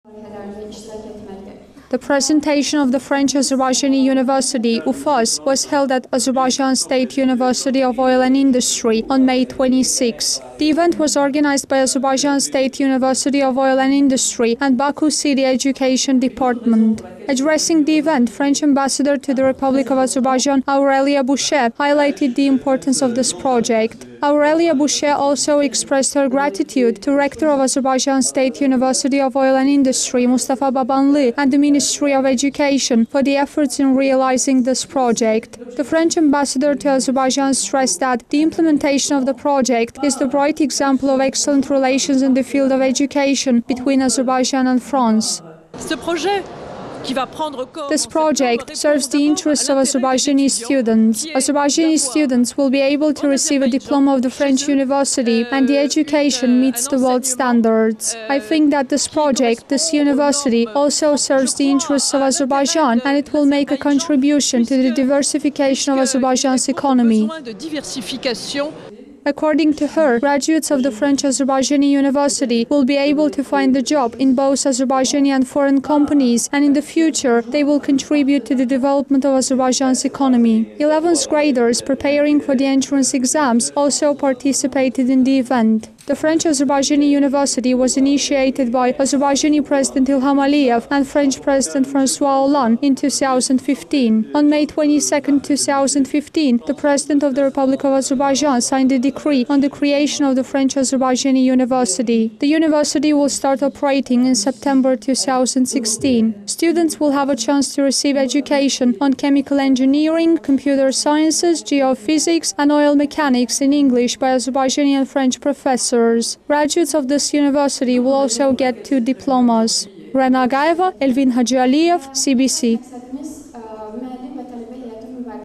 THE PRESENTATION OF THE FRENCH-AZERBAIJANI UNIVERSITY
The presentation of the French – Azerbaijani University (UFAZ) was held at Azerbaijan State University of Oil and Industry on May 26. The event was organized by Azerbaijan State University of Oil and Industry and Baku City Education Department. Eleventh graders preparing for the entrance exams also participated in the event.
Addressing the event, French Ambassador to the Republic of Azerbaijan, Aurelia Bouchez highlighted the importance of this project. Aurelia Bouchez also expressed her gratitude to rector of Azerbaijan State University of Oil and Industry, Mustafa Babanli and the Ministry of Education for the efforts in realizing this project.
The French Ambassador to Azerbaijan highlighted that the implementation of the project is the bright example of excellent relations in the field of education between Azerbaijan and France.

AURELIA BUCHEZ – FRENCH AMBASSADOR TO AZERBAIJAN

“This project serves the interests of Azerbaijani students. Azerbaijani students will be able to receive a diploma of the French university and the education meets the world standards. I think that this project, this university also serves the interests of Azerbaijan and it will make a contribution to the diversification of Azerbaijan`s economy.”

According to her, graduates of the French – Azerbaijani University will be able to find a job in both Azerbaijani and foreign companies and in the future, they will contribute to the development of Azerbaijan`s economy.
The French – Azerbaijani University was initiated by Azerbaijani President Ilham Aliyev and French President François Hollande in 2015. On May 22, 2015, the President of the Republic of Azerbaijan signed a decree on the creation of the French – Azerbaijani University.

The university will start operating in September 2016. Students will have a chance to receive education on Chemical Engineering, Computer Sciences, Geophysics and Oil-Mechanics in English by Azerbaijani and French professors. Graduates of this university will also get two diplomas.